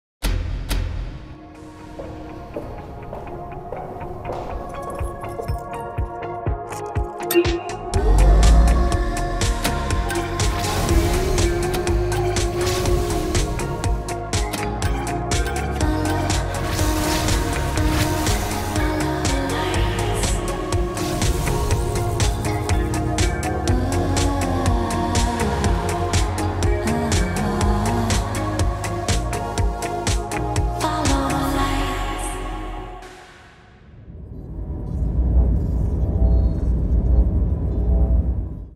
Follow the Lights to Mystic Lake
Immerse yourself in the glow of a night where the fun burns bright. All you have to do is FollowTheLights to Mystic Lake for non-stop gaming, dining, drinks, entertainment and nightlife.